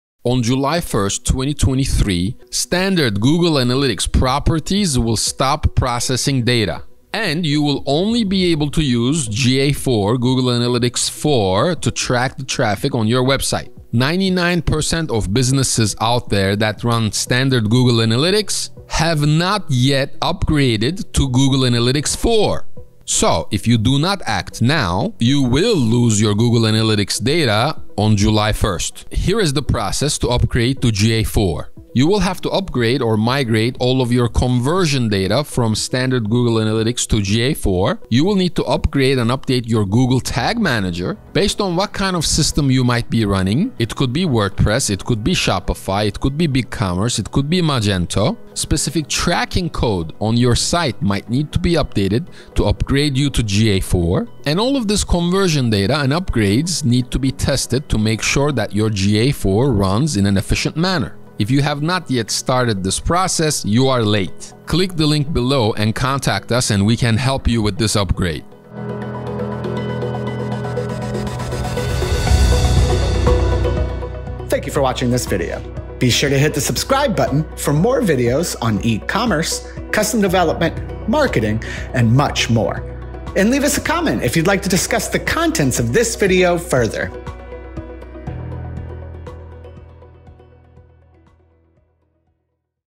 How to Upgrade to Google Analytics 4
On July 1st, 2023, standard Google Analytics properties will stop processing data and you will only be able to use GA4, Google Analytics 4, to track the traffic on your website.

99% of businesses out there that run standard Google Analytics have not yet upgraded to Google Analytics 4. So if you do not act now, you will lose your Google Analytics data on July 1st.

In this video we describe the process to upgrade to Google Analytics 4.